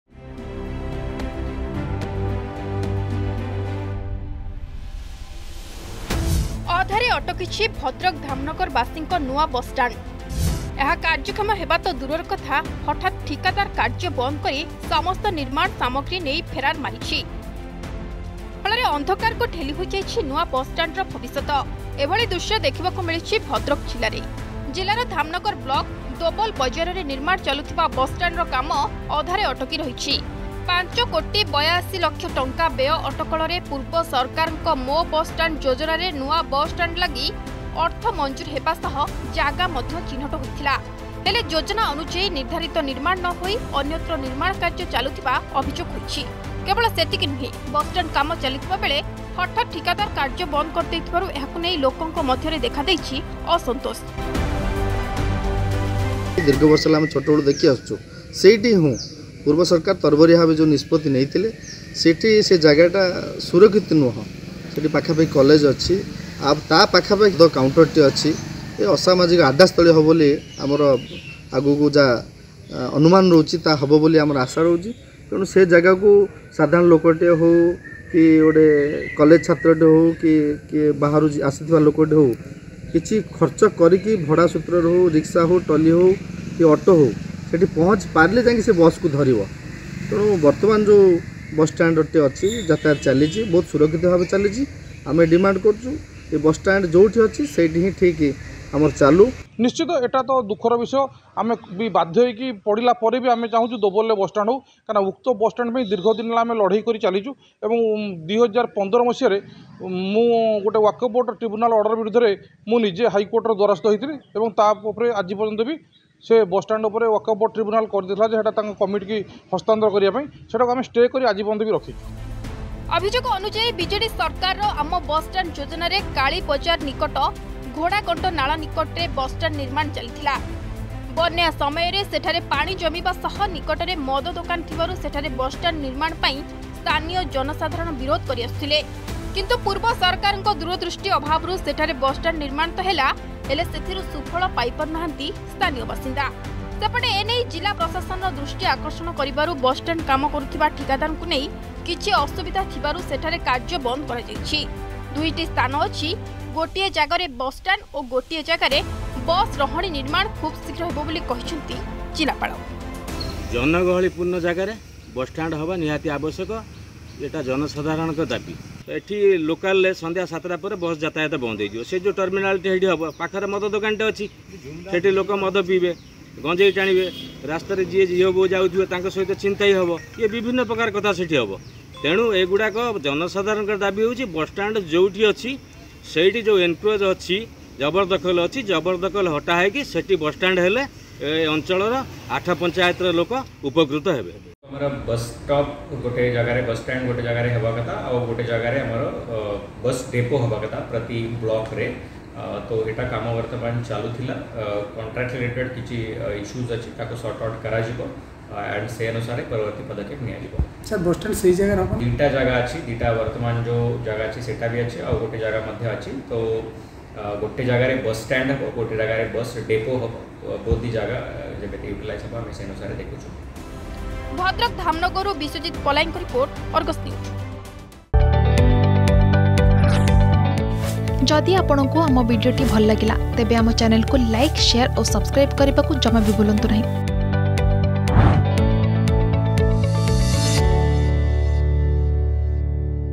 ଅଧାରେ ଅଟକିଛି ଭଦ୍ରକ ବାସୀଙ୍କ ନୂଆ ବସଷ୍ଟାଣ୍ଡ ସ୍ବପ୍ନ ।|| Bhadrak Ama Busstand
ଅଧାରେ ଅଟକିଛି ଭଦ୍ରକ ବାସୀଙ୍କ ନୂଆ ବସଷ୍ଟାଣ୍ଡ ସ୍ବପ୍ନ । ଏହା କାର୍ଯ୍ୟକ୍ଷମ ହେବା ତ ଦୂରର କଥା ହଠାତ ଠିକାଦାର କାର୍ଯ୍ୟ ବନ୍ଦ କରି ସମସ୍ତ ନିର୍ମାଣ ସାମଗ୍ରୀ ନେଇ ଫେରାର ମାରିଛି । ଫଳରେ ବସଷ୍ଟାଣ୍ଡ ଭବିଷ୍ୟତ ଏବେ ଅନିଶ୍ଚିତତା ଆଡକୁ ଠେଲି ହୋଇଯାଇଛି ।